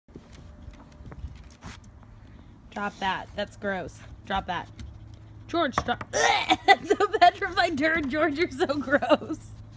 Cutest Pit Bull Ever Brings His Owner a Disgusting Surprise!
George is a super cute pit bull! And he's a GOOD boy. He loves bringing his owners gifts to say "thank you for 'dopting me!" But, George goes a little too far when he brings his owner something VOMIT-WORTHY!